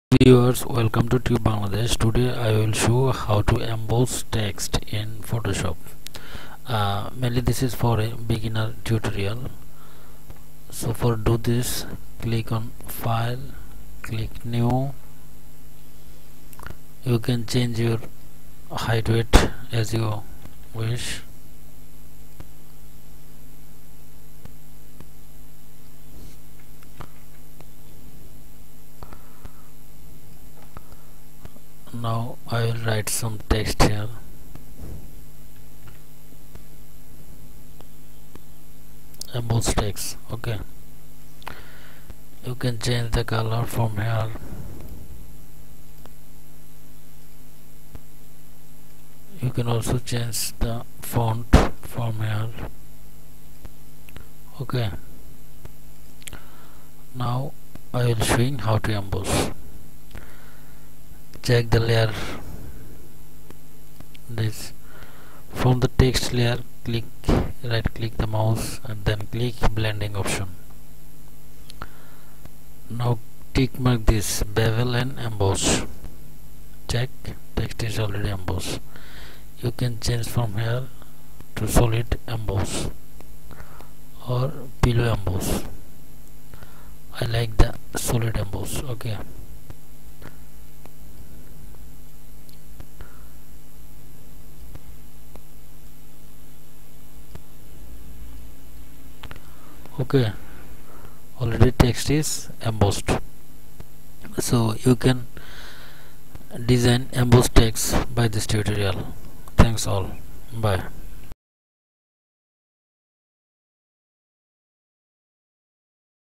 How to emboss text in photoshop for beginners
emboss  text in photoshop.
bevel and emboss text in photoshop.